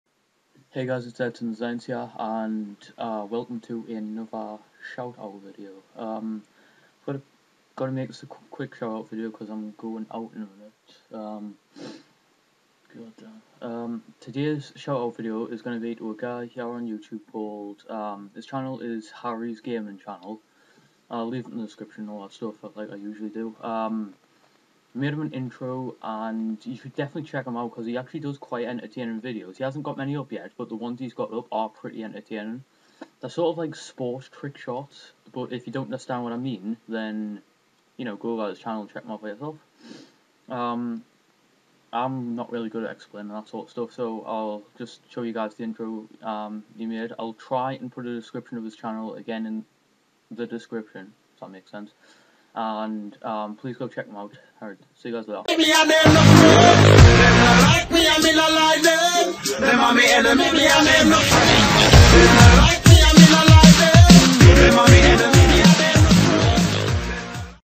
ShoutOut Video - HarrysGamingChannel - Something Different
A Brief Description Of his Channel:
He Does Trickshots With Real Life Sports E.G Table tennis/Football.
It's Entertaining to watch and something different to watch so definitely go check him out.